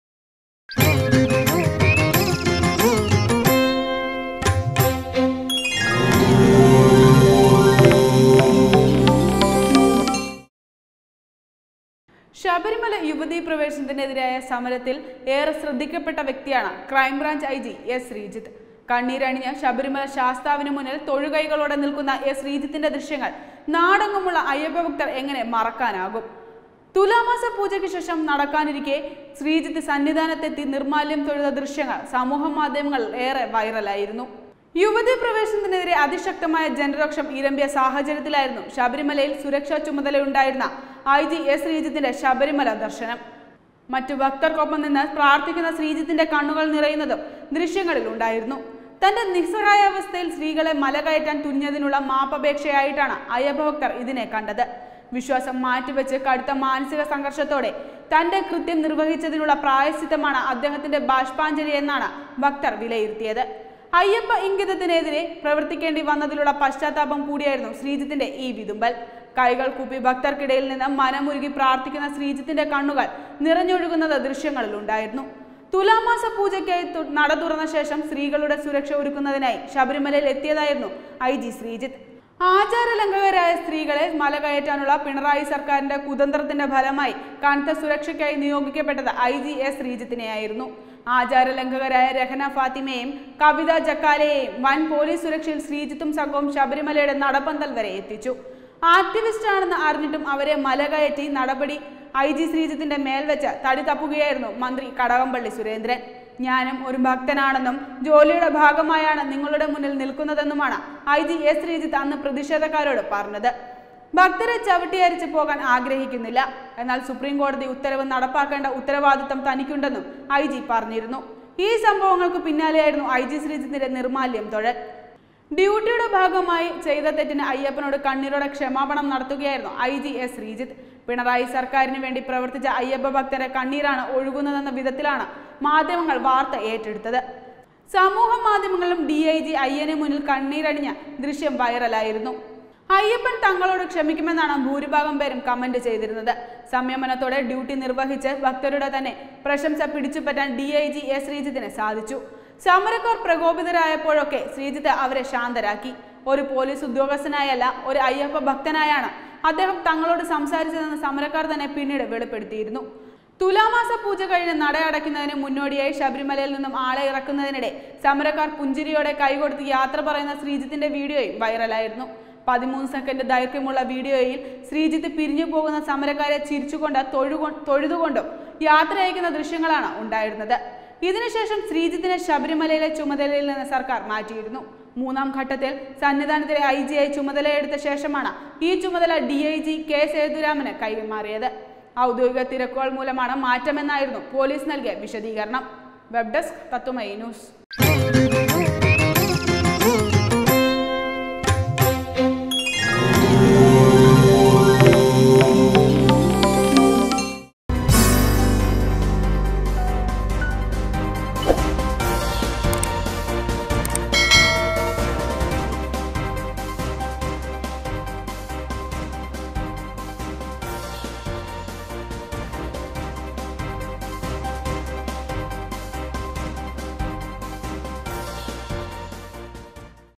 പൊന്നയ്യനേ മാപ്പ് നല്‍കണേ; ഐ ജിയുടെ പശ്ചാത്തപ കണ്ണീര്‍
ശബരിമല യുവതീപ്രവേശനത്തിനെതിരായ സമരത്തില്‍ ഏറെ ശ്രദ്ധിക്കപ്പെട്ട വ്യക്തിയാണ് ക്രൈംബ്രാഞ്ച് ഐജി എസ് ശ്രീജിത്ത്. കണ്ണീരണിഞ്ഞ് ശബരിമല ശാസ്താവിനു മുന്നിൽ തൊഴുകൈകളോടെ നിൽക്കുന്ന  എസ് ശ്രീജിത്തിന്‍റെ ദൃശ്യങ്ങള്‍ നാടെങ്ങുമുള്ള അയ്യപ്പഭക്തര്‍ക്ക് എങ്ങനെ മറക്കാനാകും. തുലാമാസ പൂജയ്ക്ക് ശേഷം നടയടക്കാനിരിക്കെ ശ്രീജിത്ത് സന്നിധാനത്തെത്തി നിര്‍മാല്യം തൊഴുത ദൃശ്യങ്ങള്‍ സമൂഹമാധ്യമങ്ങളില്‍ ഏറെ വൈറലായിരുന്നു.